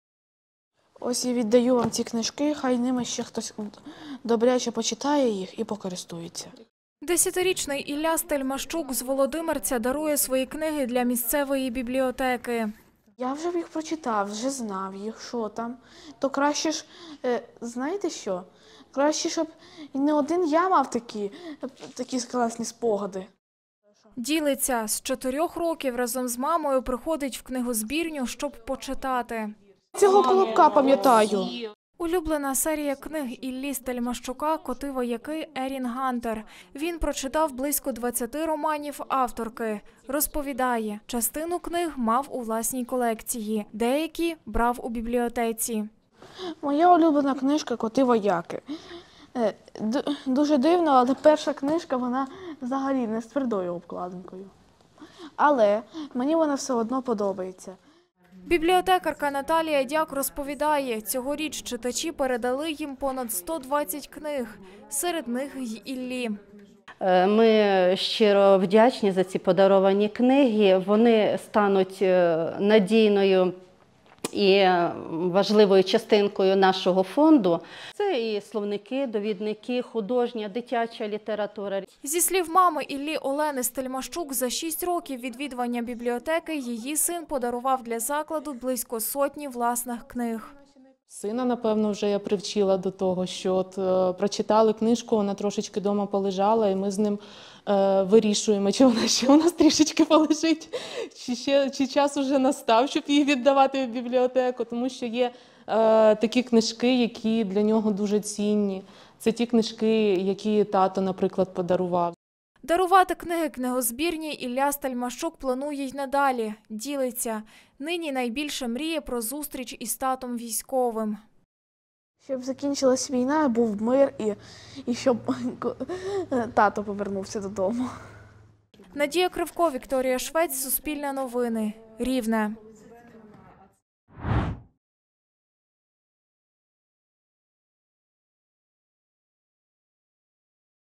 П'ятикласник дарує книги для місцевої бібліотеки, щоб їх могли прочитати інші діти
У Володимирці на Рівненщині десятирічний Ілля Стельмащук упродовж декількох років передає прочитані книги місцевій бібліотеці. Робить це, щоб інші могли безплатно прочитати більше літератури. Книги передає разом з мамою.